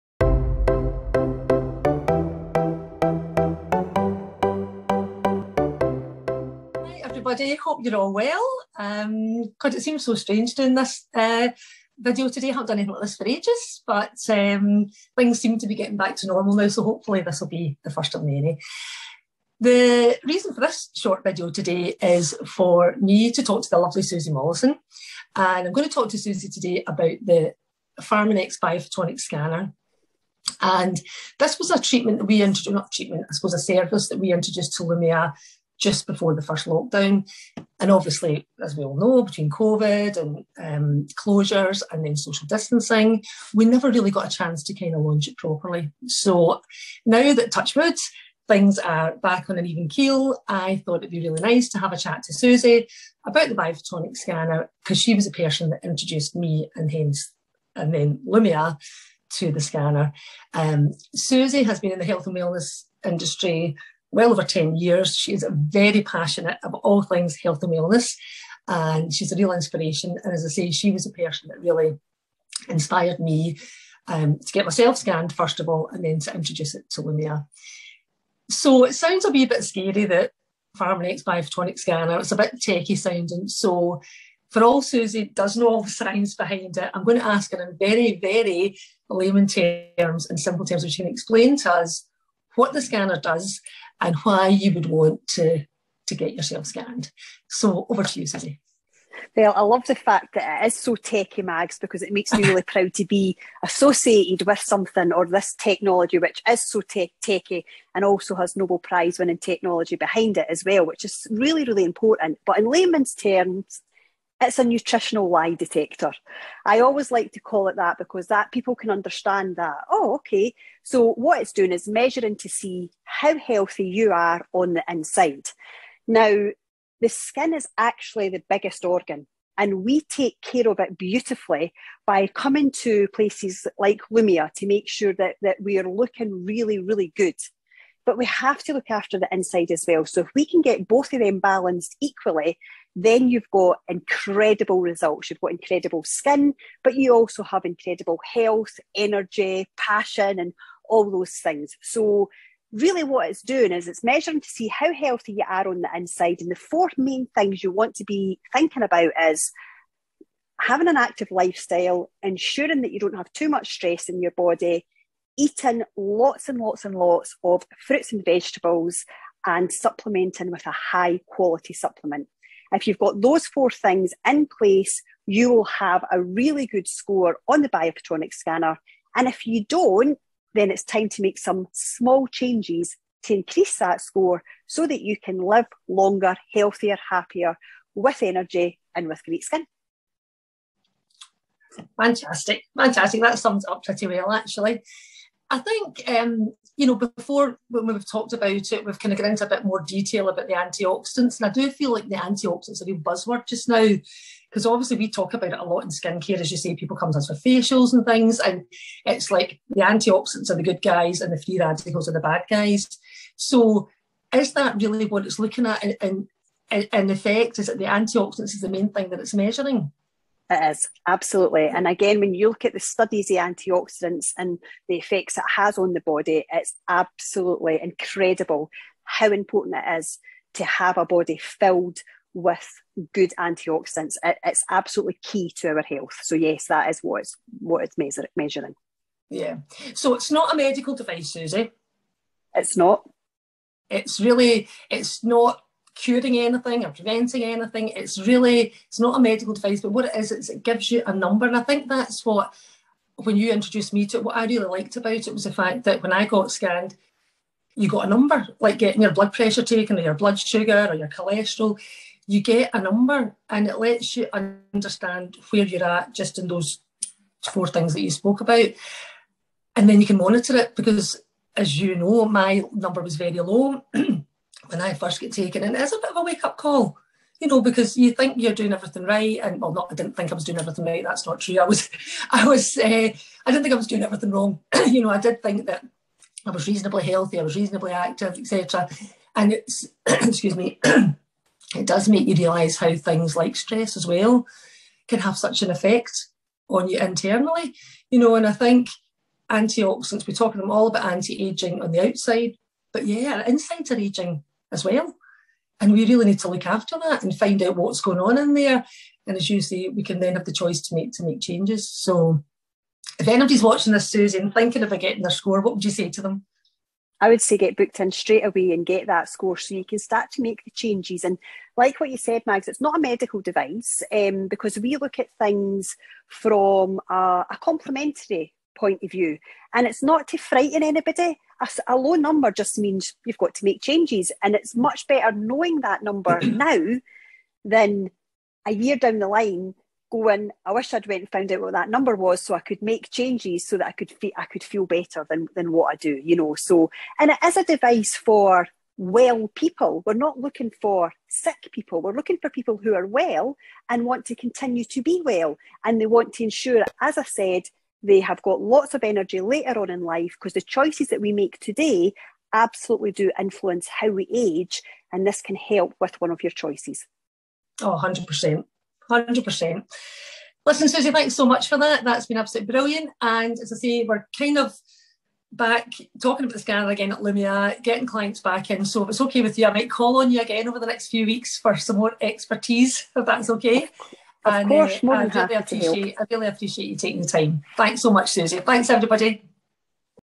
Healthy Ageing at Lumia, Vlog 1 - Ageing from the Inside Out
Mags chats with Susie Mollison about healthy ageing and how you can find out what’s going on in the inside.